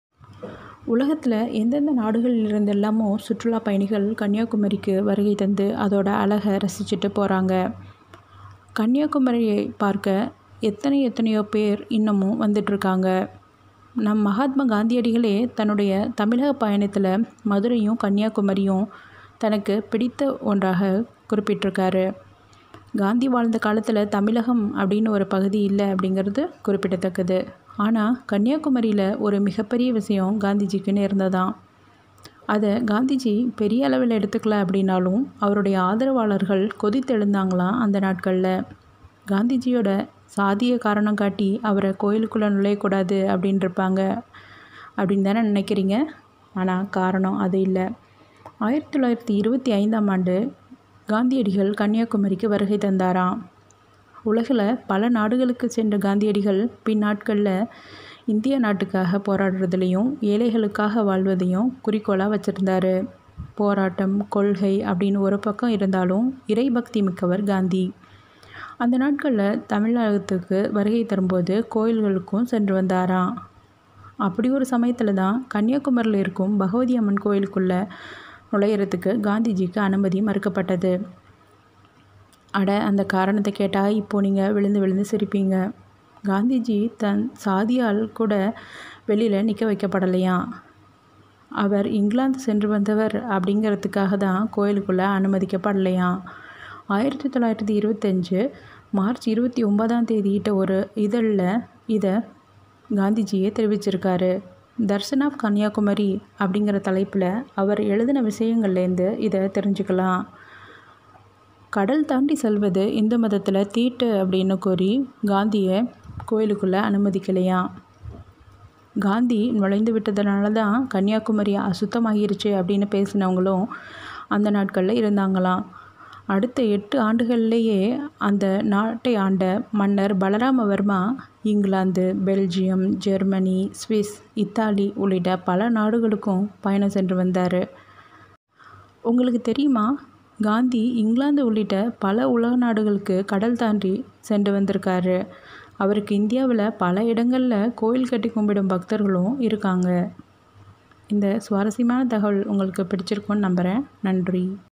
காந்தி நுழைய அனுமதி மறுத்த கோவில் பற்றி தெரியுமா?
Surprising and interesting life incidents of Mahathma,.